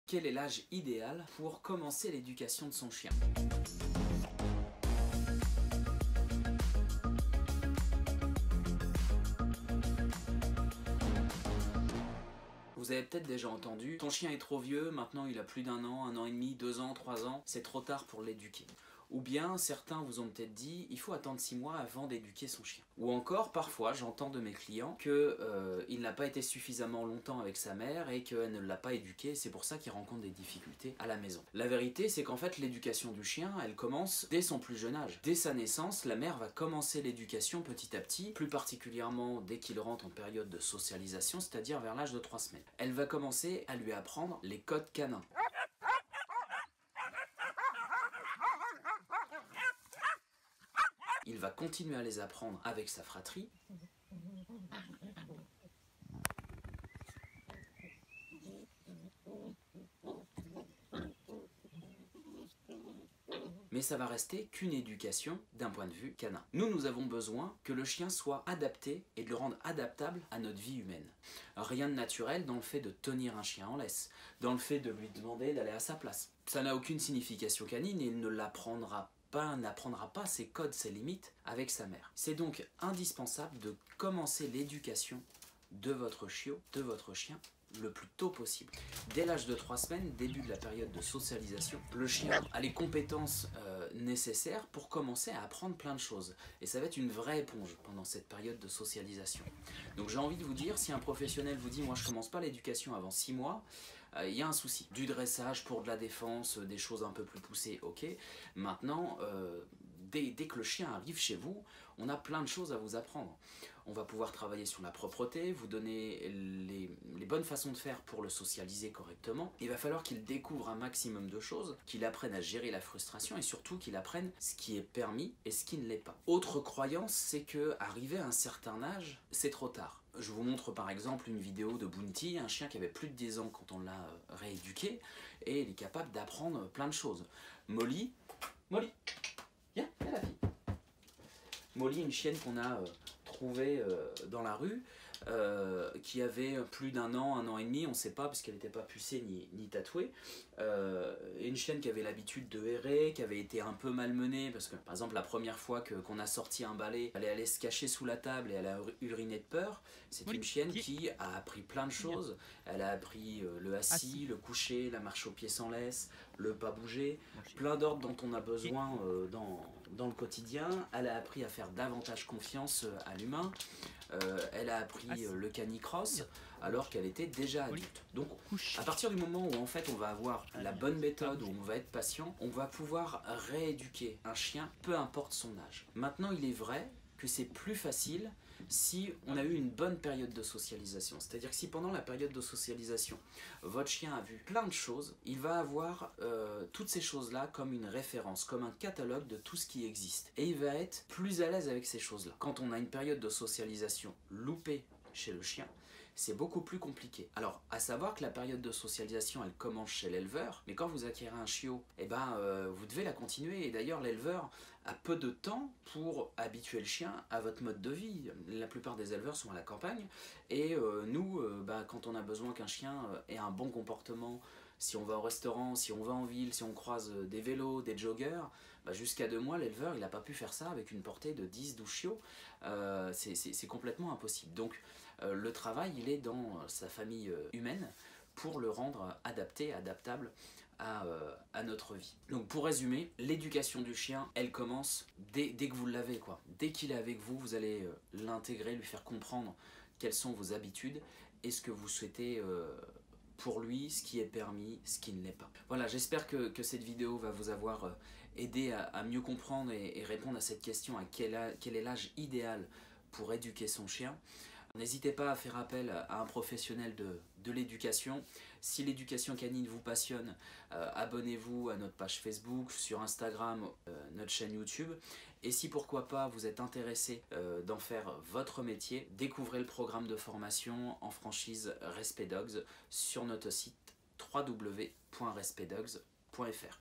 A QUEL ÂGE EDUQUER SON CHIOT ?
Quand commencer l'éducation de son chien ?
Trop jeune, trop vieux on ne sait plus quoi penser Pourtant vous avez surement envie et besoin de lui apprendre plein de choses
Découvrez dans cette vidéo la réponse à vos questions sur l'âge pour éduquer votre chiot ou chien.

Recevez votre e-book gratuit en vous inscrivant ainsi que du contenu exclusif pour éduquer votre chien !